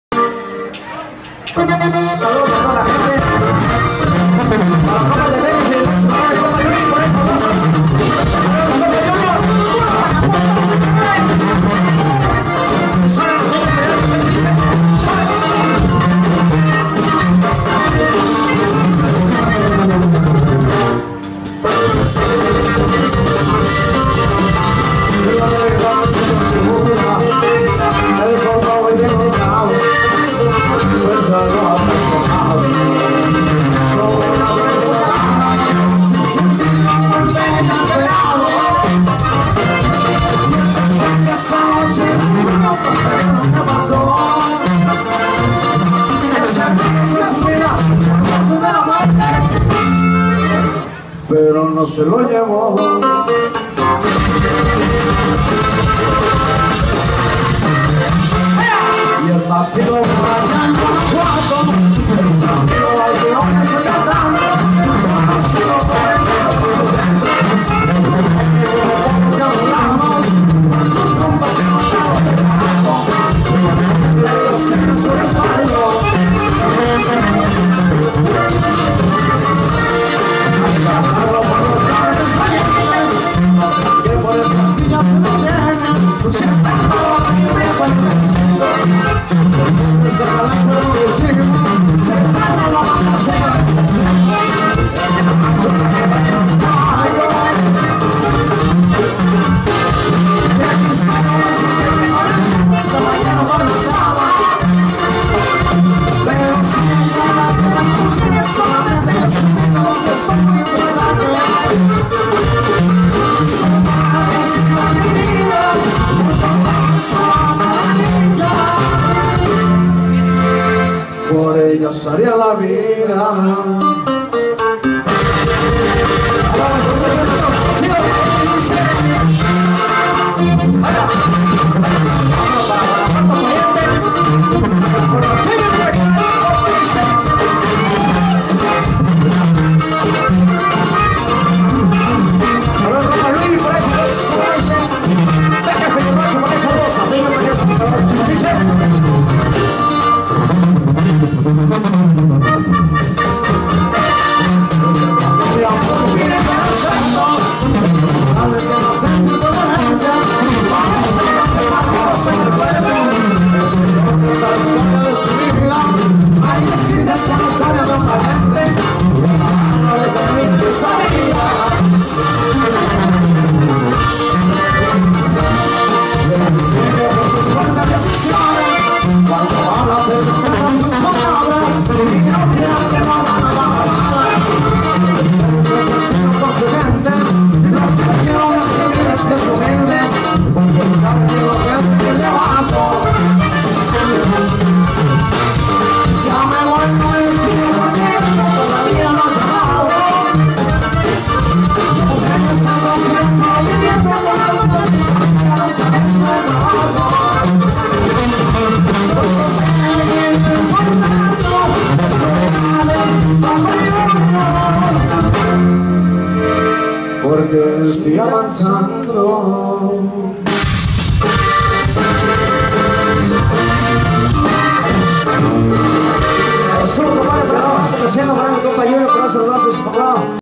Norteño imperial en bakersfield CA
En la movida night club abriendo el concierto para Fidel rueda